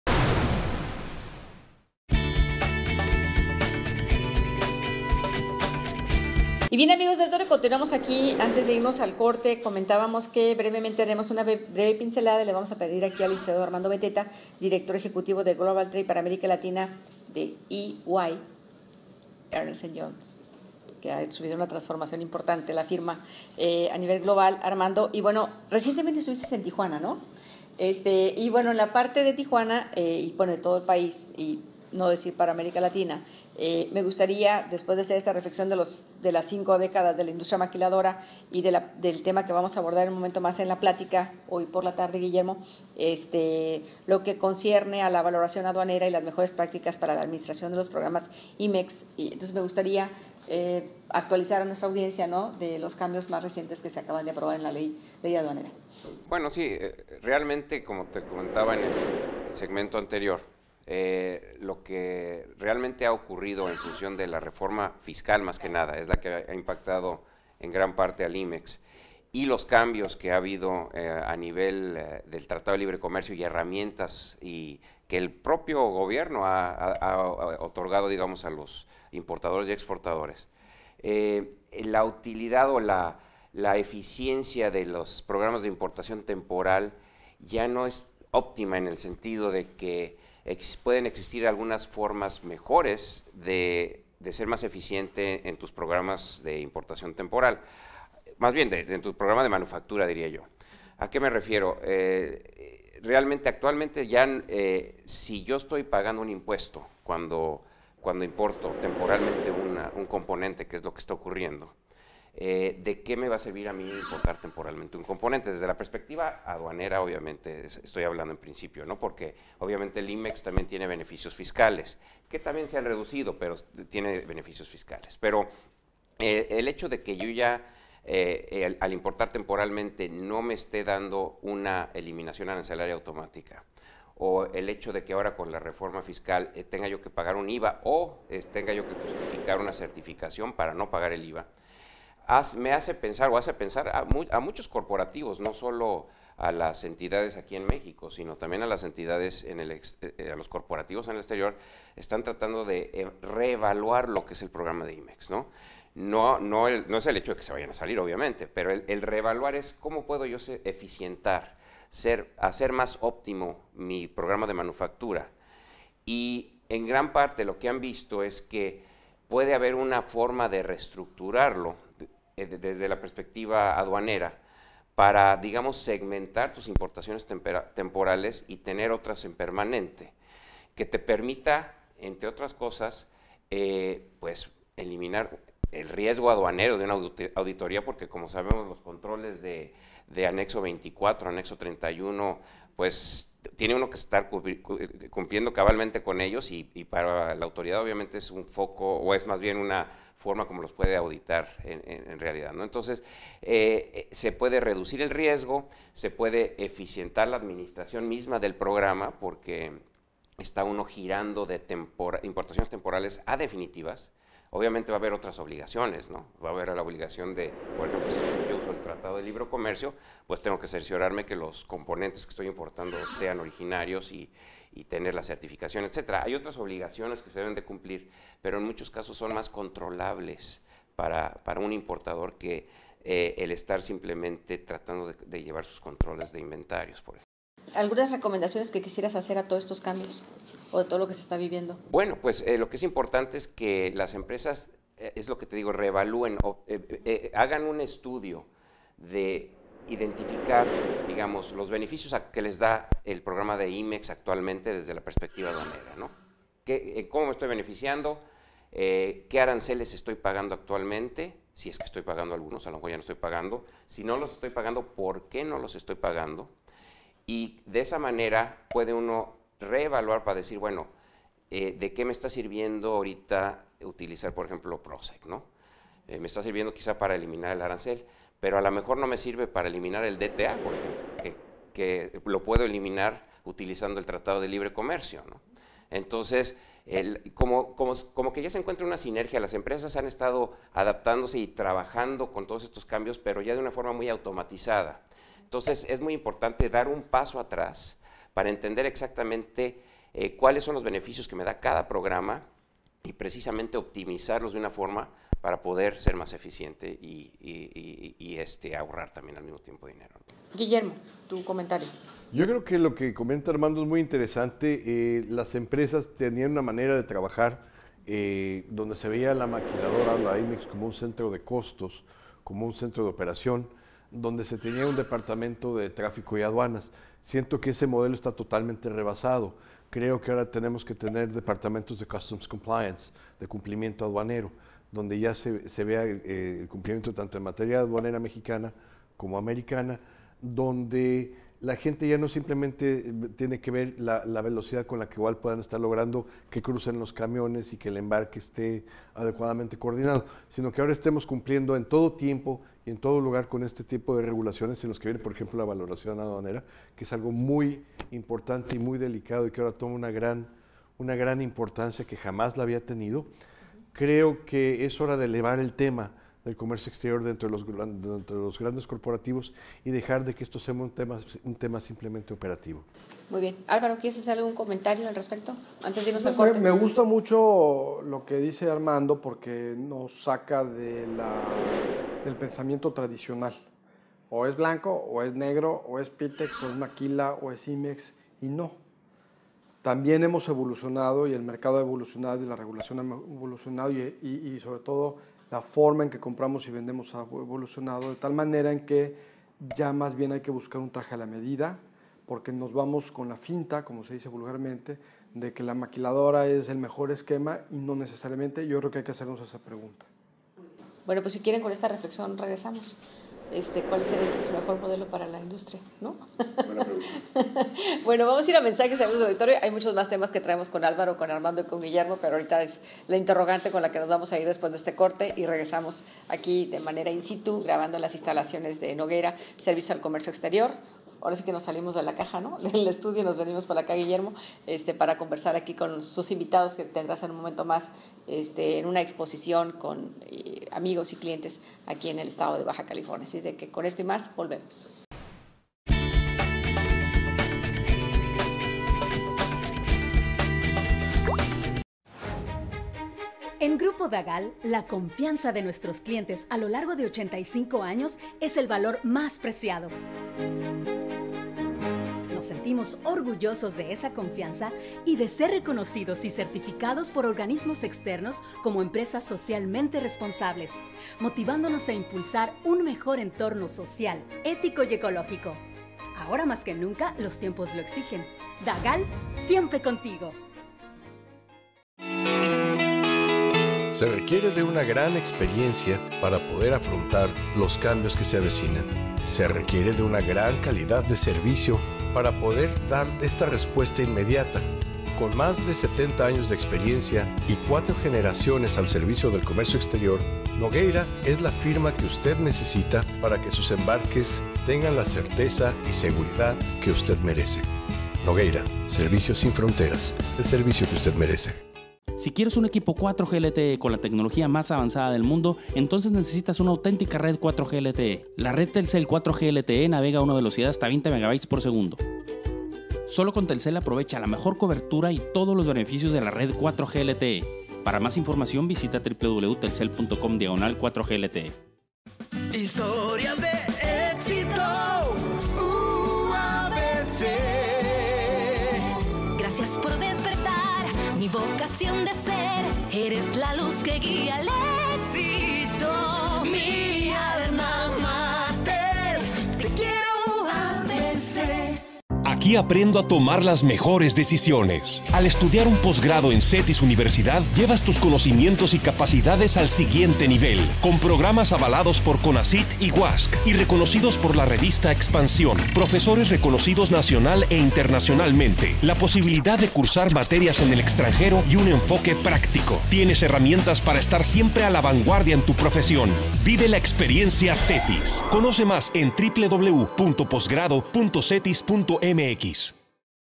Ultimas novedades en comercio exterior
Tercer segmento con la participación desde Nogueira Agencia Aduanal con la participación de Guillermo Nogueira, Fundador de Nogueira Servicios al Comercio Exterior, Álvaro Quintana E., Asesor en Comercio Exterior y Ex Director General de aduanas y Armando Beteta C. Dir Ejecutivo de Global Trade para Latinoamérica en Ernest & Young